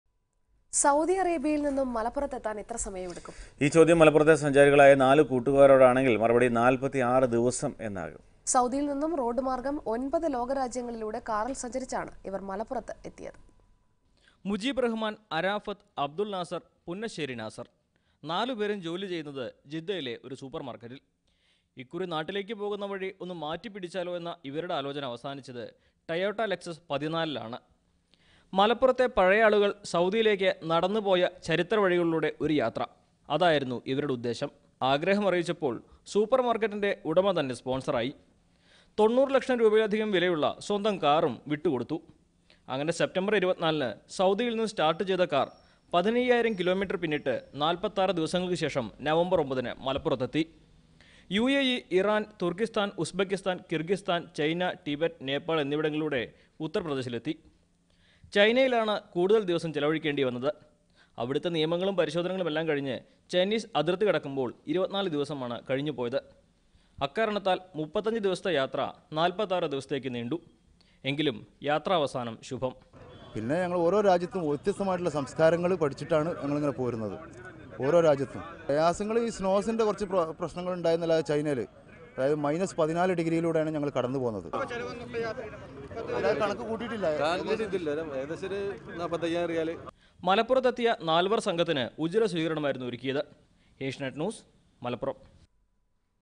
Gulf Malayali team travelled Jeddah to Malappuram by road: Gulf News
Gulf Malayali team which travelled  here from Saudi Arabia after travelling thousands of kilometres by road on Sunday